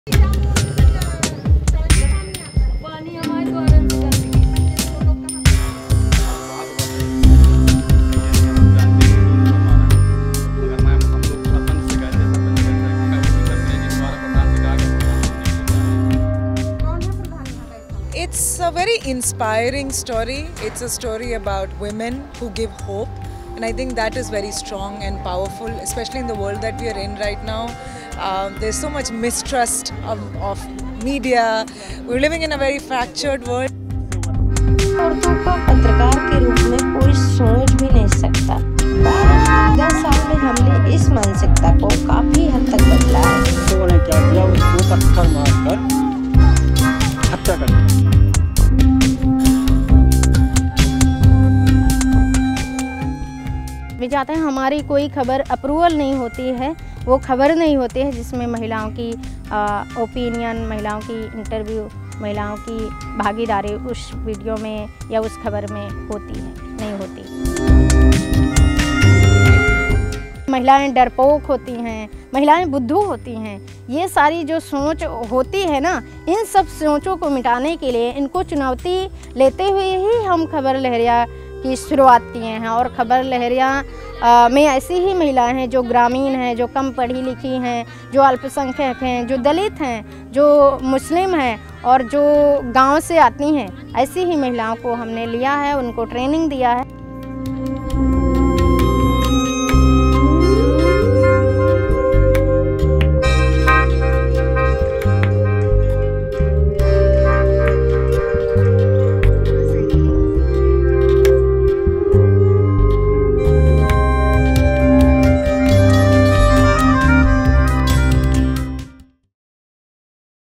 Indian documentary "Writing with fire" nominated for academy awards 2022
اتر پردیش کی خواتین صحافیوں پر بنی ڈاکومینٹری آسکر کے لیے نامزد